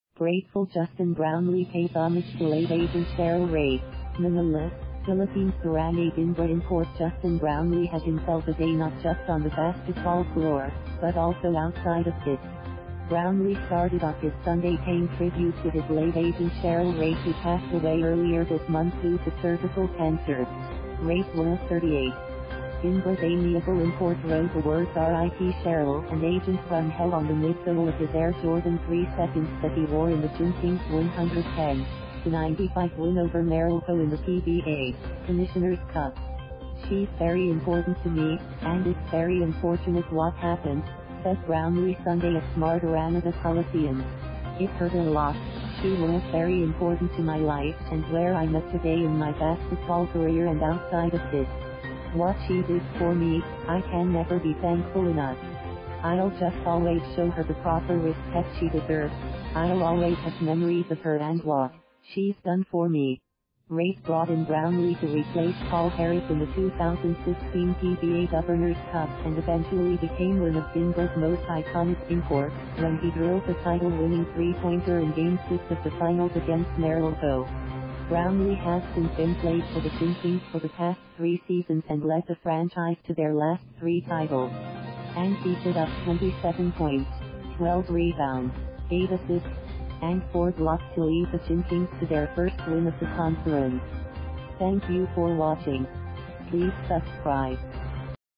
Grateful Justin Brownlee pays homage to late agent Sheryl Reyes
MANILA, Philippines—Barangay Ginebra import Justin Brownlee had himself a day not just on the basketball floor, but also outside of it. Brownlee started off his Sunday paying tribute to his late agent Sheryl Reyes who passed away earlier this month due to cervical cancer. Reyes was 38. Ginebra’s amiable import wrote the words “RIP Sheryl” and “Agent from hell” on the midsole of his Air Jordan 3s that he wore in the Gin Kings’ 110-95 win over Meralco in the PBA Commissioner’s Cup. “She’s very important to me, and it’s very unfortunate what happened,” said Brownlee Sunday at Smart Araneta Coliseum. “It hurt a lot, she was very important to my life and where I’m at today in my basketball career and outside of it.” “What she did for me, I can never be thankful enough. I’ll just always show her the proper respect she deserves, I’ll always have memories of her and what she’s done for me.” Reyes brought in Brownlee to replace Paul Harris in the 2016 PBA Governors’ Cup and eventually became one of Ginebra’s most iconic imports when he drilled the title winning three-pointer in Game 6 of the finals against Meralco. Brownlee has since then played for the Gin Kings for the past three seasons and led the franchise to their last three titles. And he put up 27 points, 12 rebounds, eight assists, and four blocks to lead the Gin Kings to their first win of the conference.